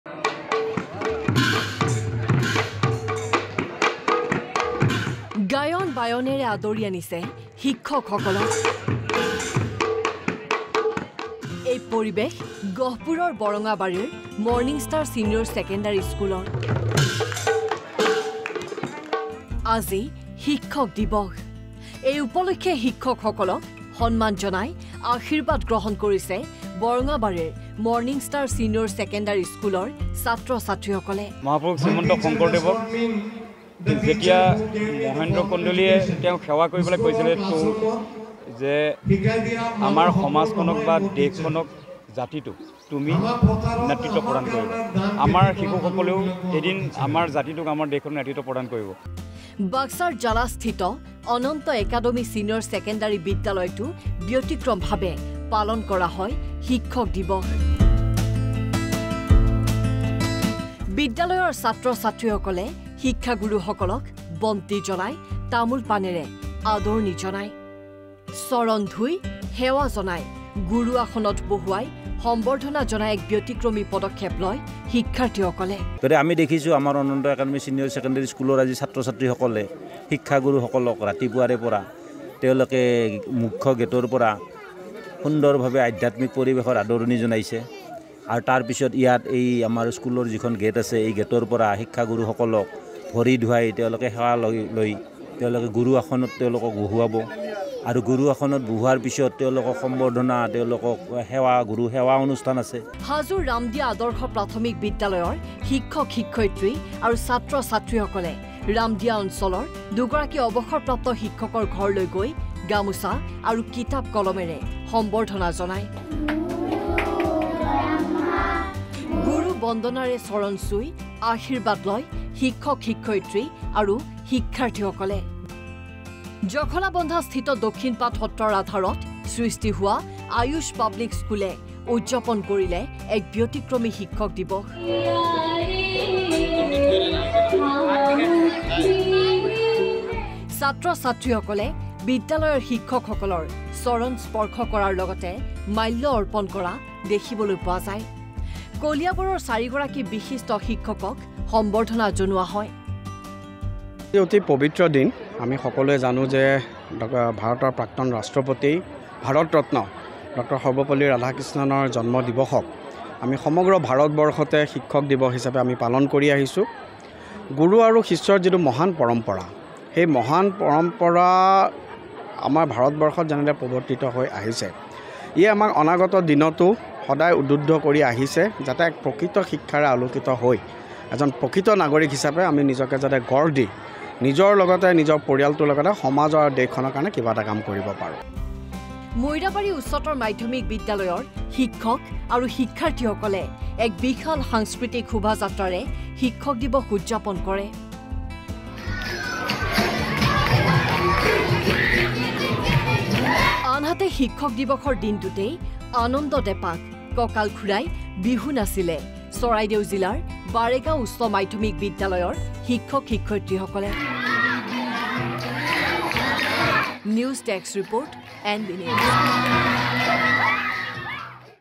শিক্ষক দিৱস পালন। গায়ন-বায়নেৰে আদৰি আনিছে শিক্ষকসকলক
শিক্ষক দিৱস পালন। গায়ন-বায়নেৰে আদৰি আনিছে শিক্ষকসকলক। এই পৰিৱেশ গহপুৰৰ বৰঙাবাৰীৰ মৰ্ণিংষ্টাৰ ছিনিয়ৰ ছেকেণ্ডাৰী স্কুলৰ।